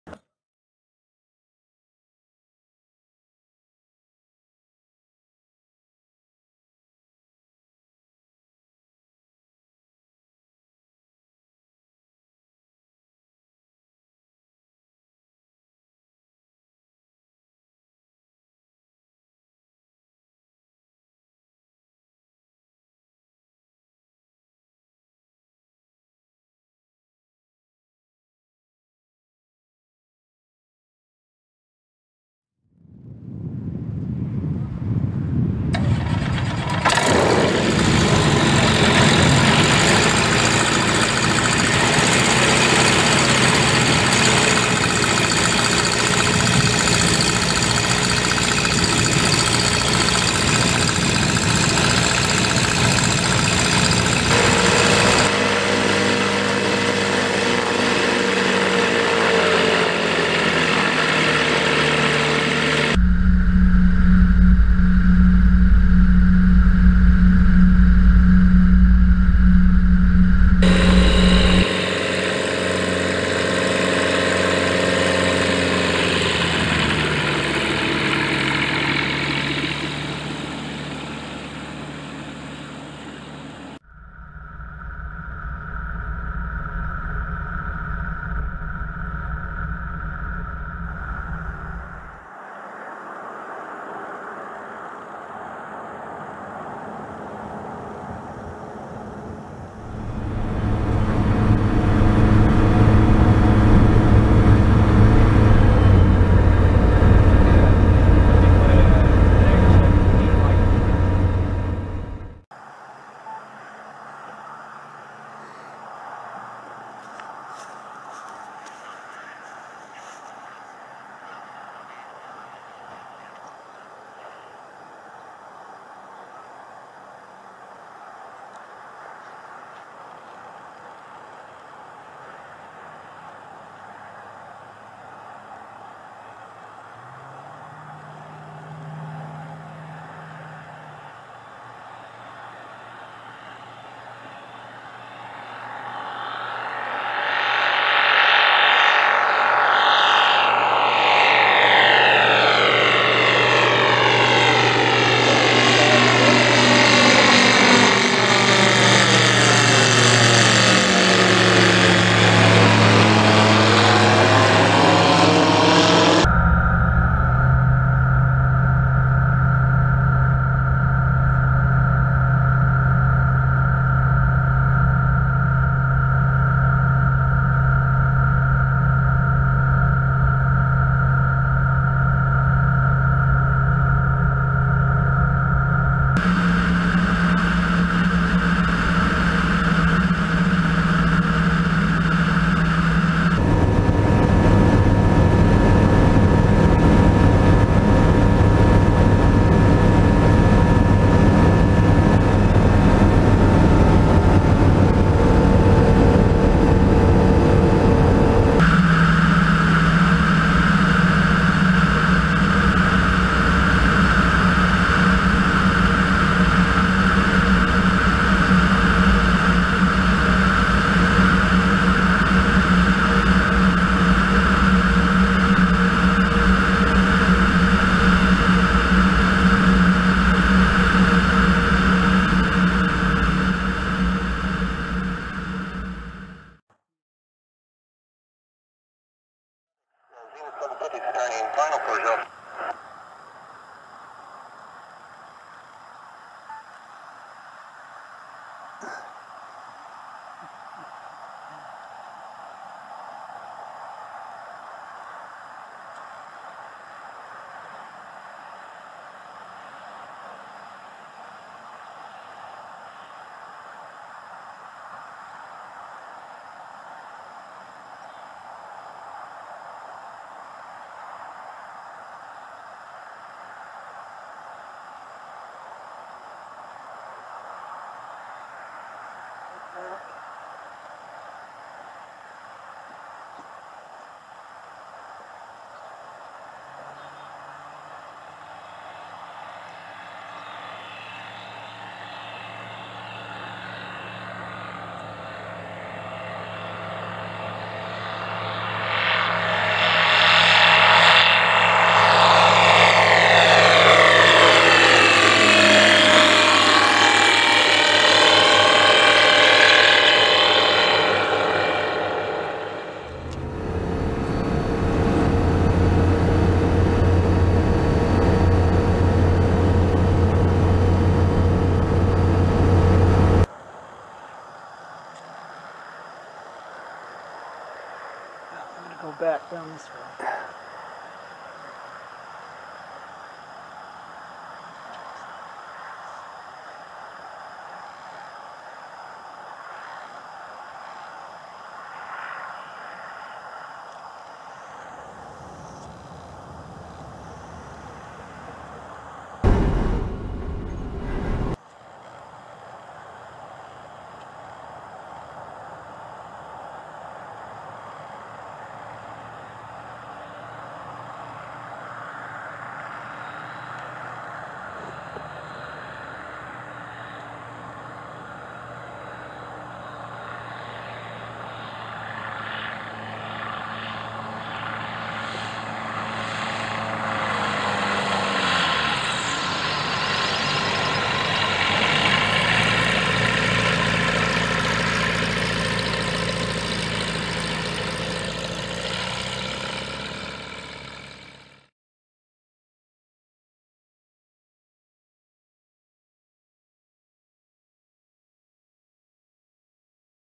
First Flight: Zenith STOL CH 750 with the Rotax 912iS engine
First Flight: Zenith STOL CH 750 with the Rotax 912iS engine, custom built by Skytek Aircraft Services in Vernon, BC, Canada.
First flight performed by Zenith Aircraft Company's Roger at Vernon Regional Airport, home of Rotech Research Canada Ltd., the exclusive distributor of Rotax Aircraft Engines in Canada.